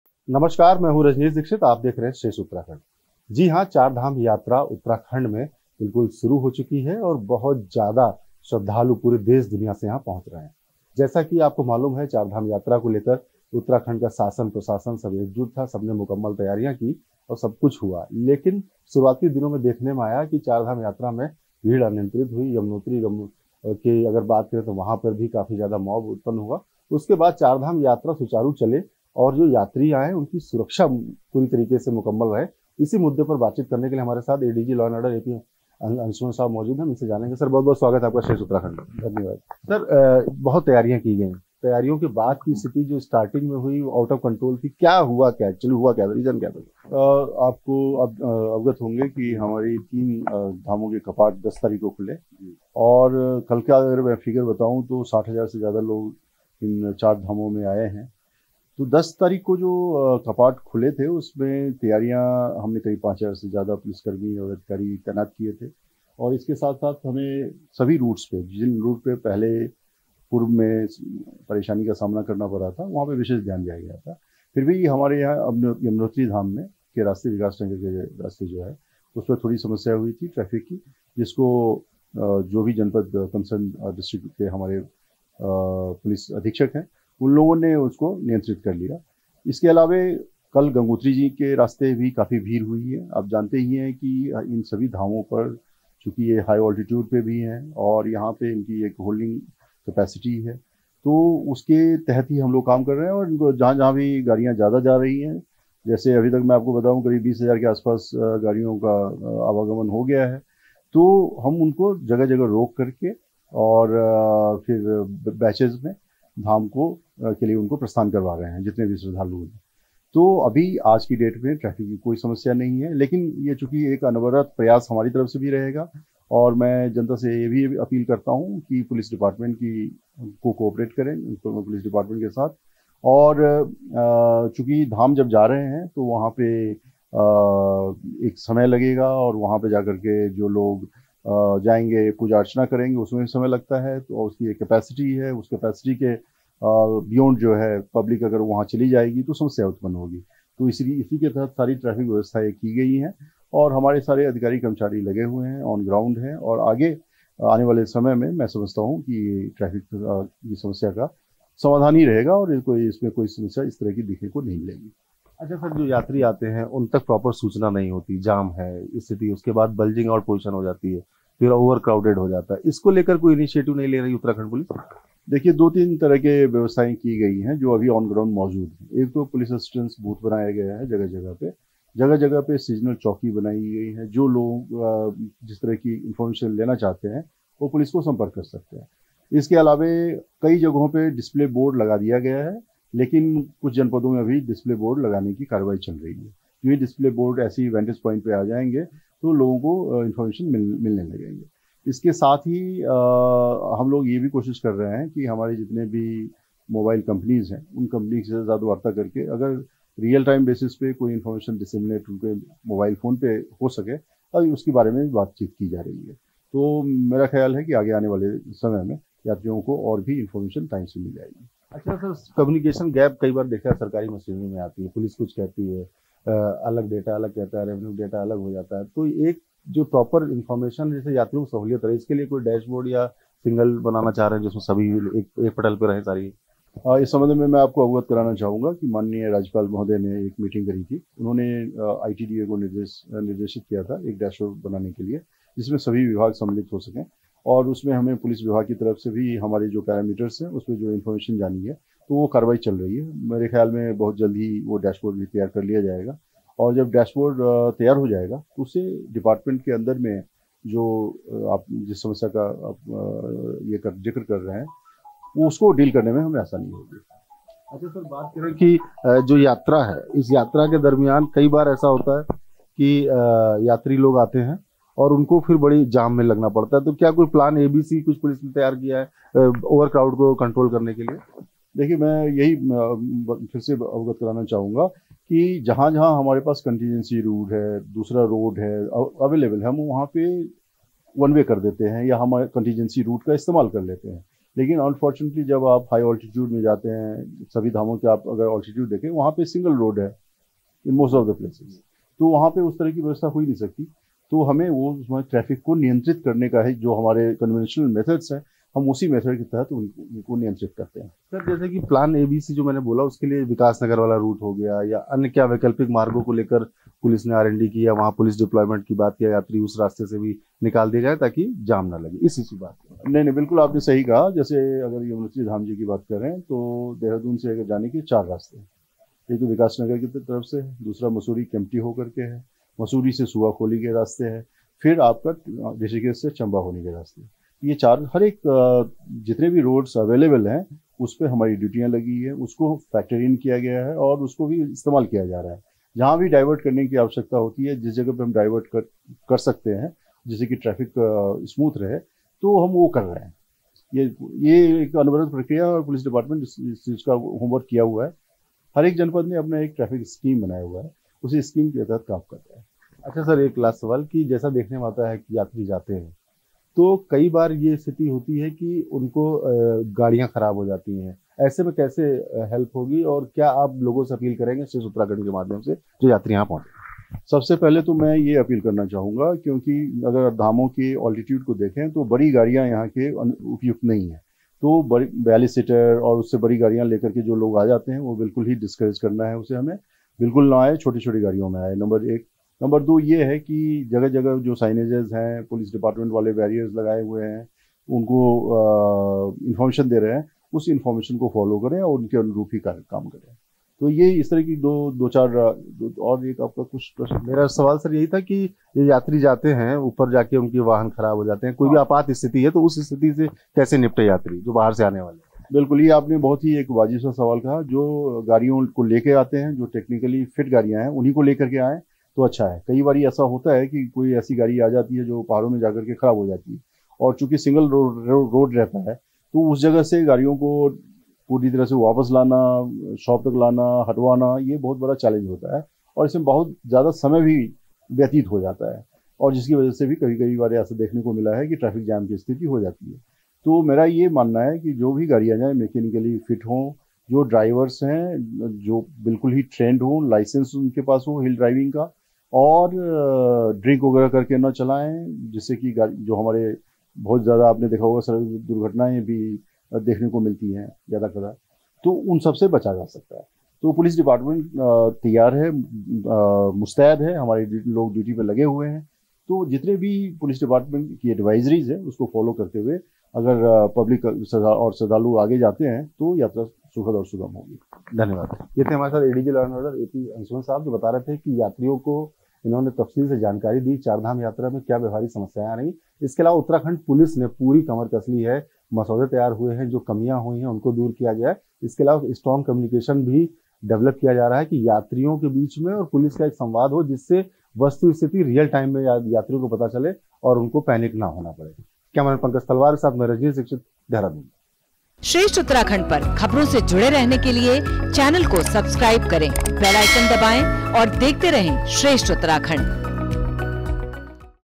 Char Dham Yatra 2024: ADG (Law and Order) AP Anshuman से श्रद्धालुओं की समस्याओं पर बातचीत Top News
Char Dham Yatra 2024: ADG (Law and Order) AP Anshuman से श्रद्धालुओं की समस्याओं पर बातचीत Top News | Uttarakhand News

चारधाम शुरू होते ही यमुनोत्री- गंगोत्री में जाम की भारी समस्या देखने को मिली है. आने वाले समय में यात्रा सुचारू रूप से कैसे चलेगी. इस हमारे संवाददाता रजनीश दीक्षित ने ADG एपी अंशुमान से बातचीत की. इस दौरान ADG ने यात्रा से जोड़े तमाम मुद्दों पर कई मु्ख्य बातों को बताया.